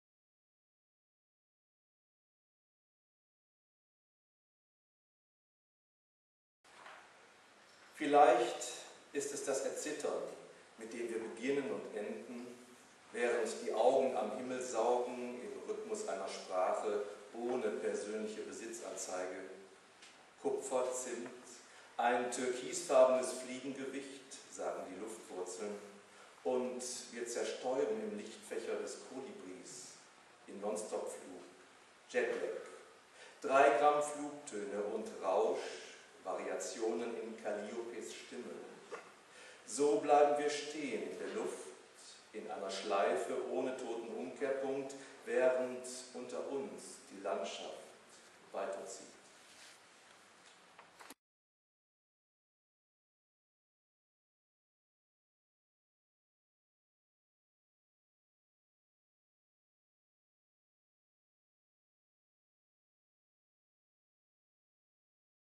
JÜRGEN NENDZA I Die Rotation des Kolibris
Jürgen Nendza (Essen, 1957), lebt in Aachen. Er veröffentlichte bislang sieben Gedichtbände, zuletzt im Verlag Landpresse die Gedichtbände 'Die Gelegenheit der Wiese / The Chance of the Meadow'(2007) und den Gedichtband 'Die Rotation des Kolibris' (2008).
Seine Gedichte wurden mehrfach ausgezeichnet, u.a. mit dem Lyrikpreis Meran.